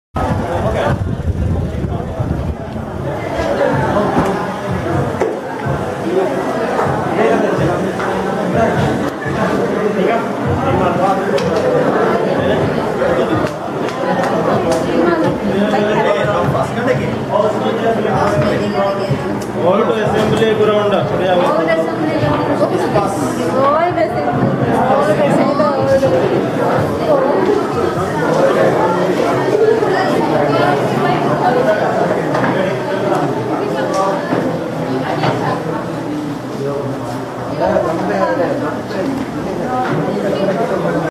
Rasoolaayah furahsaara kurumaa dhekolhah ihavandhoo gai muzaaharaa kurumah thahyaaru vany
Rasoolaayah furahsaara kurumaa dhekolhah ihavandhoo gai muzaaharaa kurumah thahyaaru vany (Oct 4, 2012)